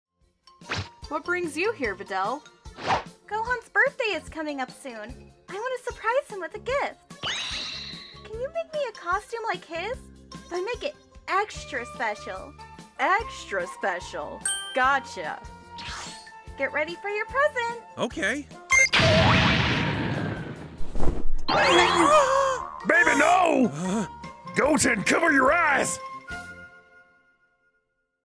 Goodness Greatious Saiyaman | DBZ Comic Dub
This comic was made by the very talent Funsexydb

Voice of Android 18: (mbteddygram)

What did you all think of Goodness Greatious Saiyaman before watching this video?

And would you wanna see more videos like Goodness Greatious Saiyaman